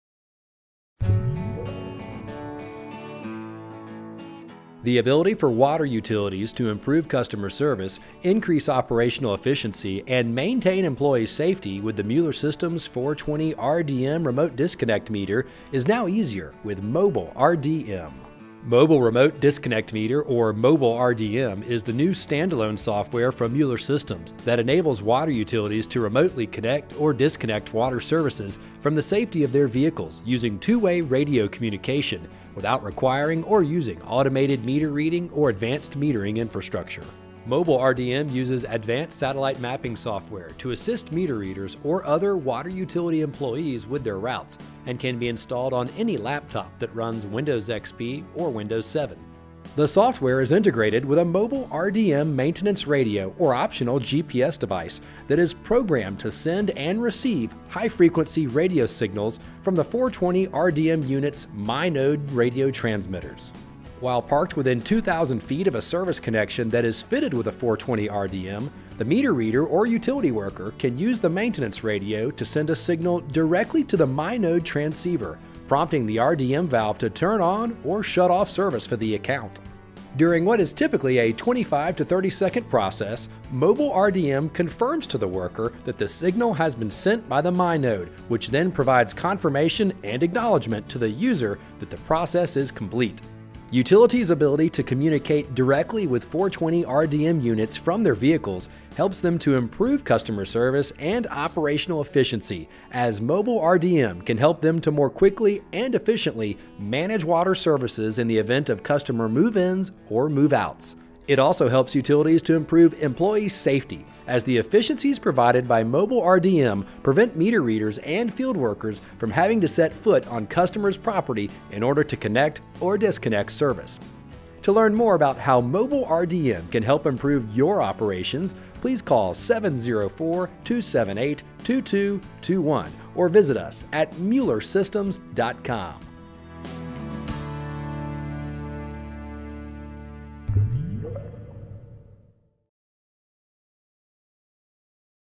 Mobile Remote Disconnect Meter from Mueller Systems
Mobile RDM is a standalone software that enables water utilities to remotely connect or disconnect water services from the safety of their vehicles using 2-way radio communication— without requiring or using advanced metering infrastructure (AMI) or automated meter reading (AMR)